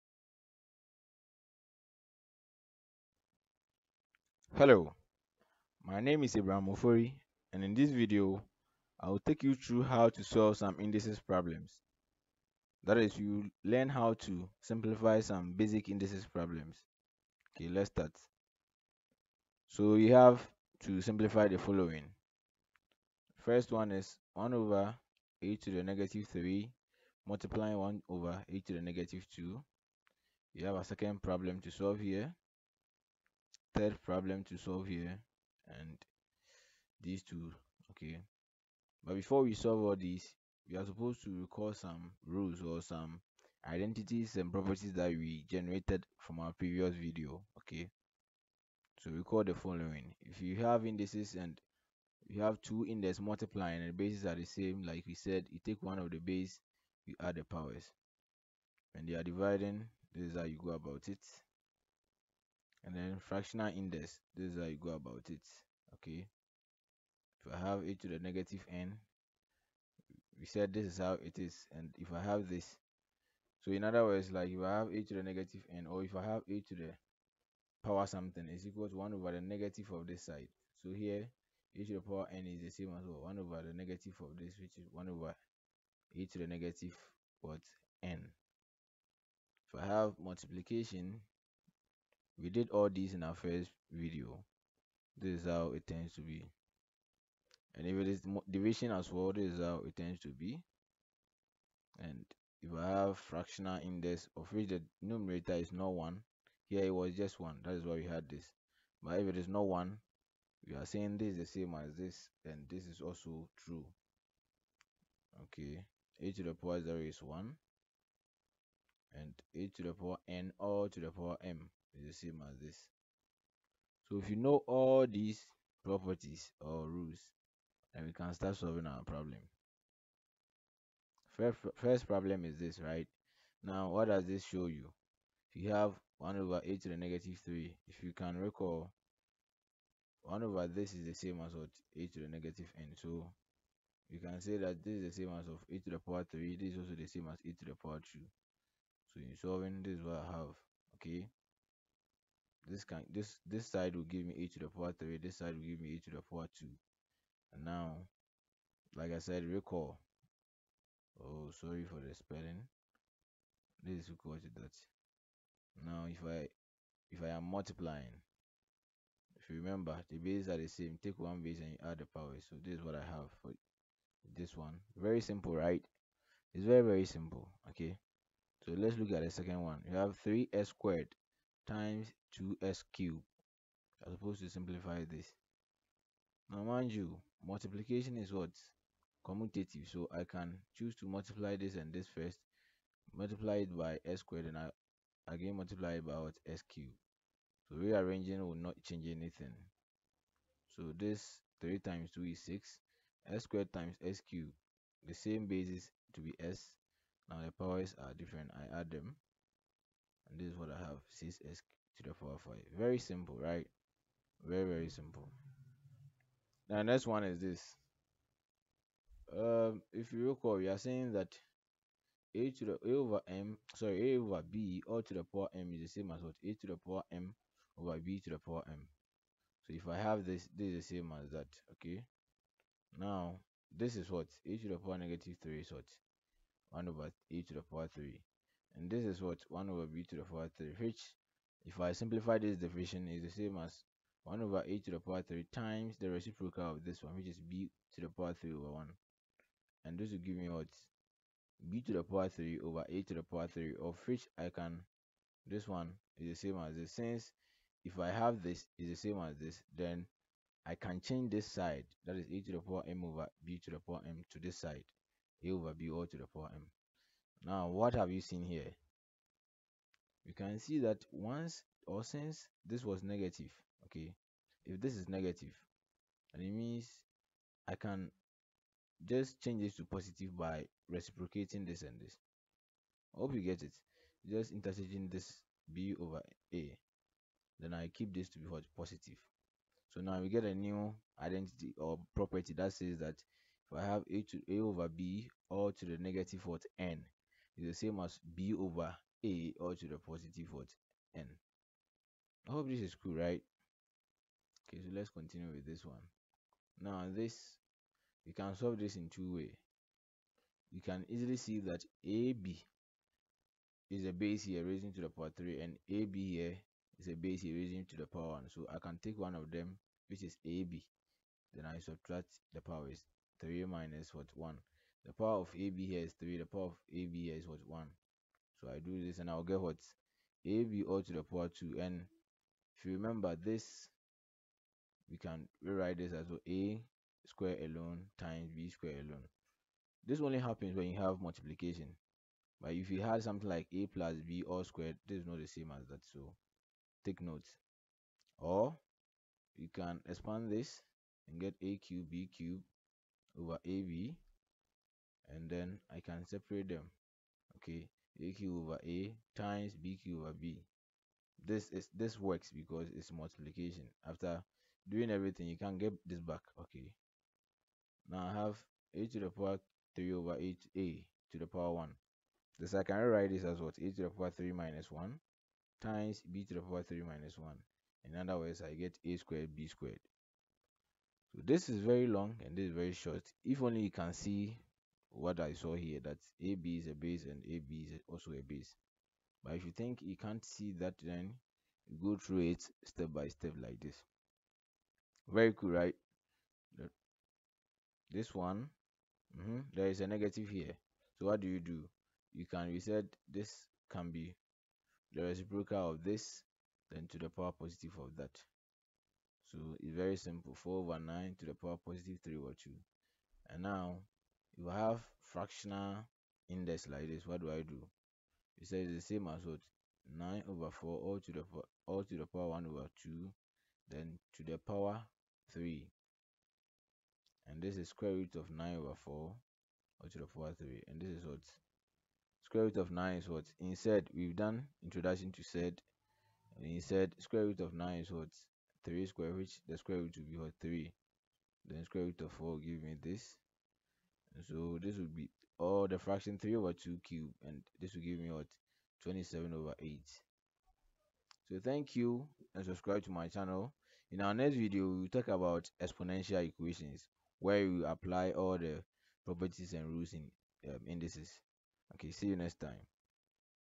Indices Examples
This video takes you through some worked examples on indices using the Properties of indices by MEXAMS